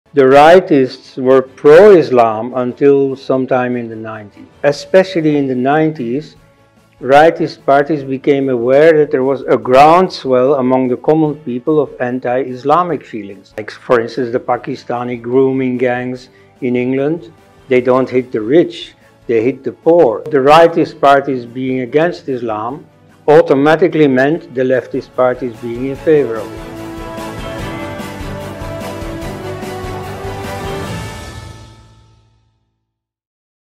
PROMO 4 | Dr. Koenraad Elst on 'How world favours Islam' [HIGHLIGHTS]
In conversation with Dr. Koenraad Elst;

A glimpse of how Dr. Elst has analysed the emphasis of both the Leftists and the Rightists & their shift from Christianity to Islam